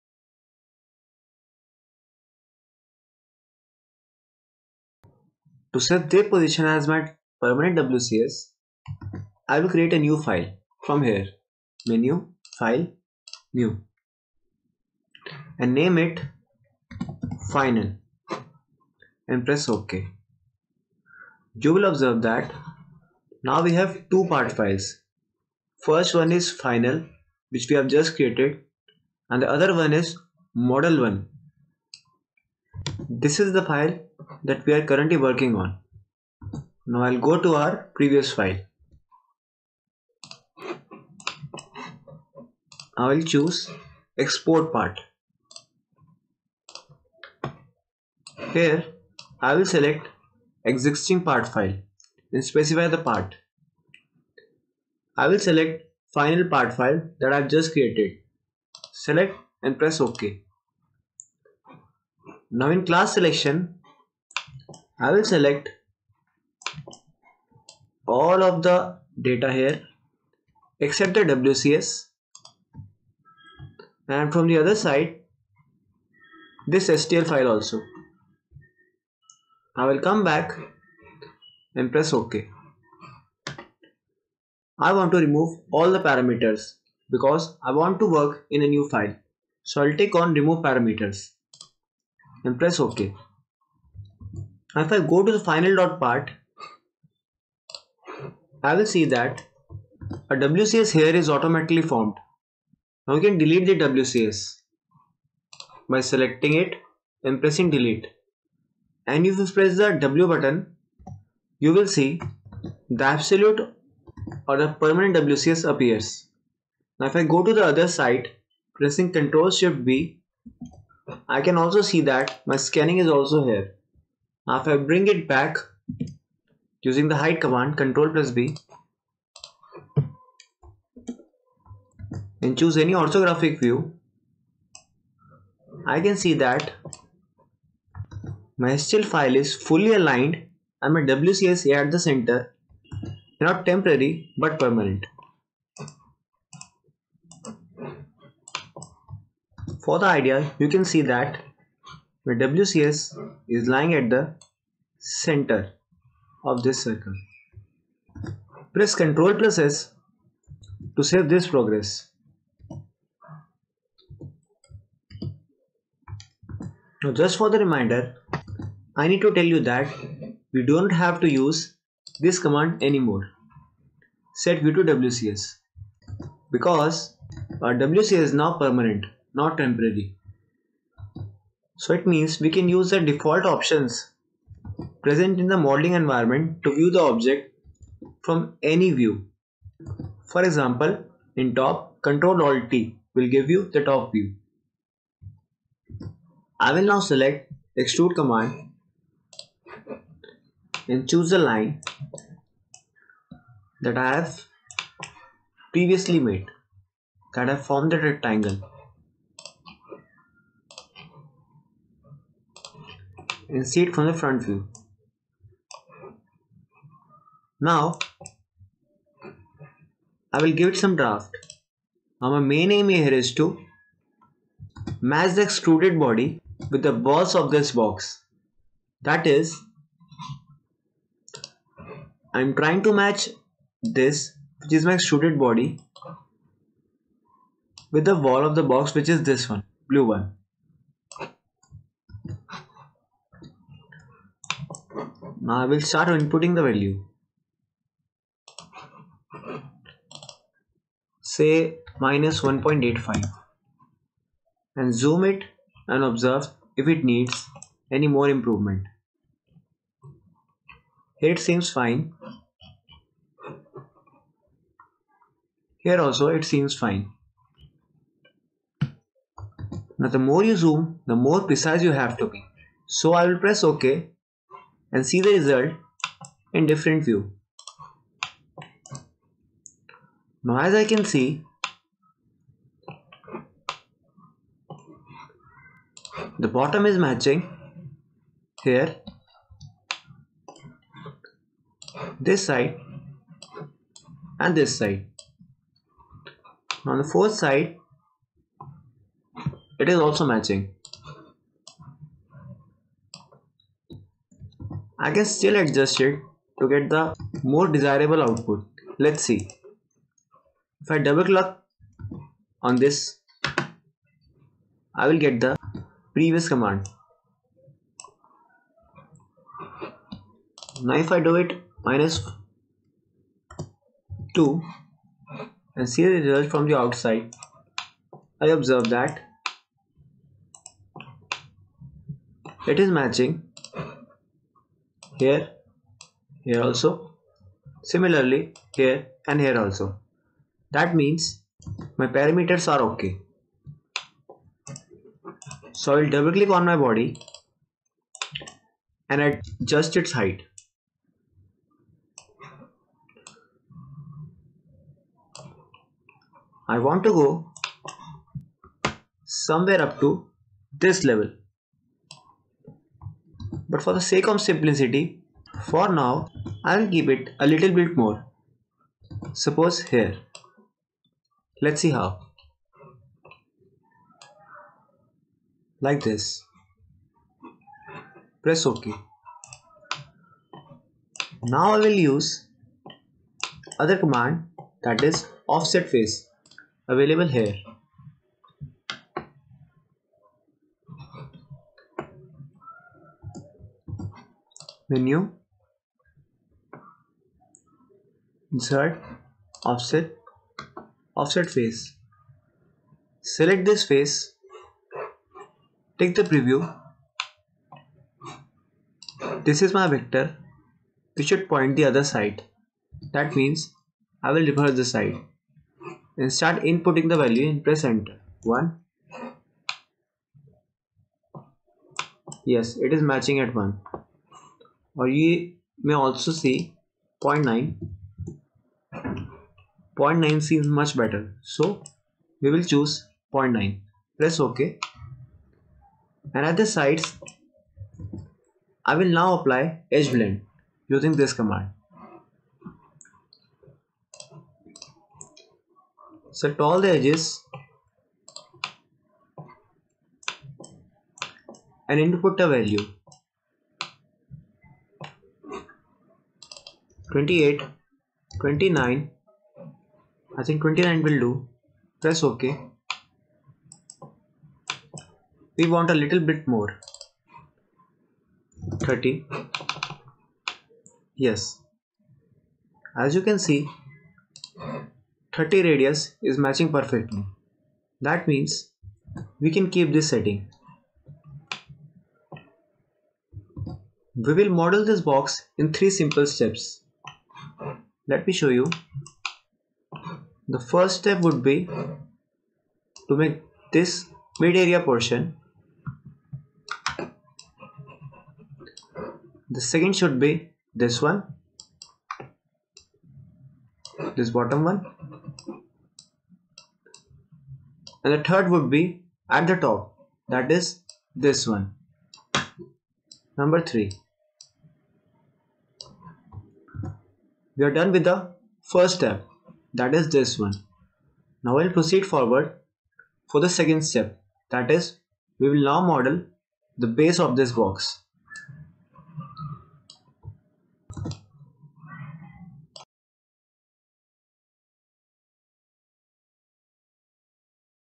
ENGLISH | 5. (Part 5/7) Extrude Command On An STL File - Reverse Engineering Course
In this tutorial we'll start the modeling of the box from the middle portion using extrude command.